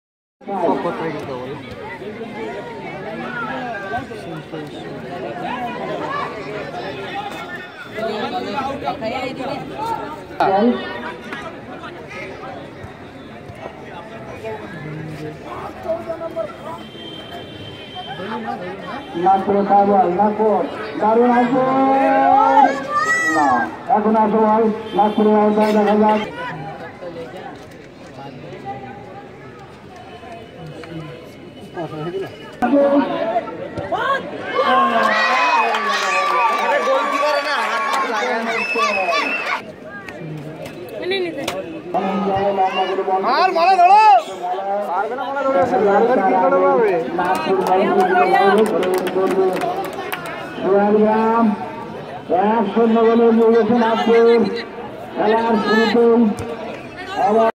Netaji Cup 17 Match of Subhashgram Vs Nagpur
Subhashgram Is Home team and Nagpur is Very Strong team & they Netji cup 17 Runner Team